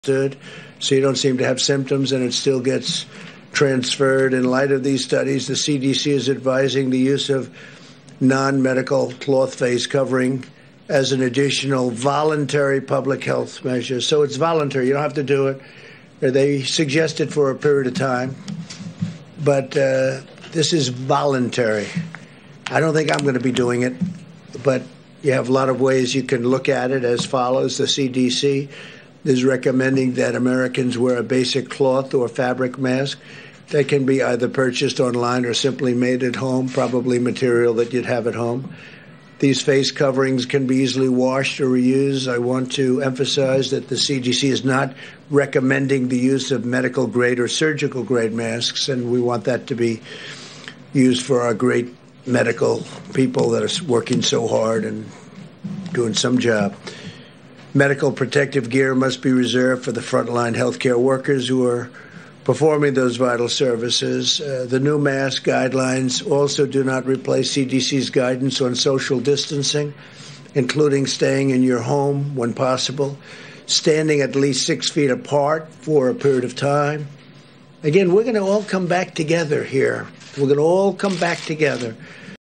Trump: The CDC Is Announcing The Use Of Non-Medical Cloth Face Covering As A Voluntary Measure
BREAKING: President @realDonaldTrump says the CDC is announcing the use of non-medical cloth face covering as an additional VOLUNTARY public health measure!